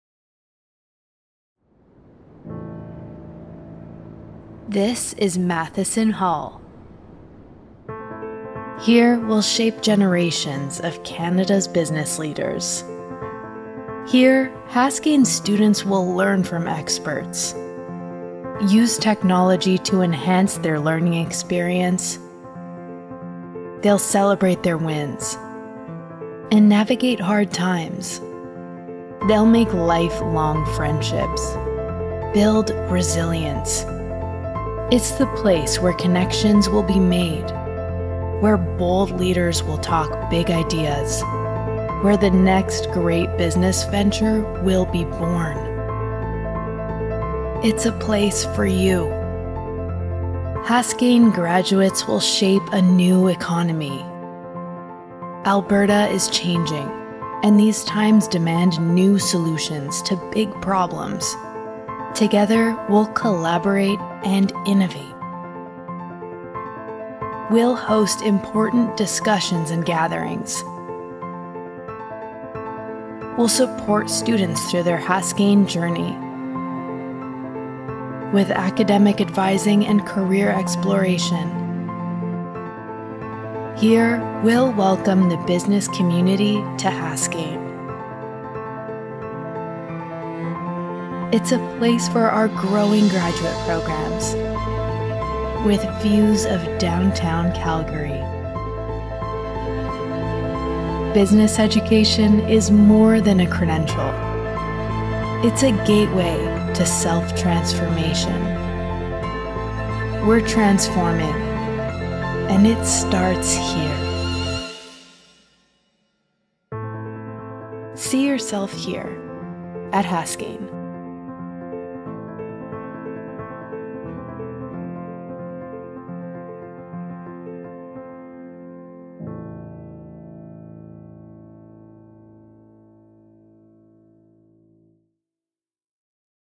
A look into Mathison Hall
The Haskayne School of Business is growing with the addition of a second building, Mathison Hall. It will be four-storeys, 10,000 m2 and connected to Scurfield Hall, our current home. In all, 12 new classrooms will be added to the business school, including a new finance lab and a 100-person theatre. The new building is set to be completed at the end of 2022. Construction has progressed considerably over the last 18 months and we’re eager to share this September 2021 update with you.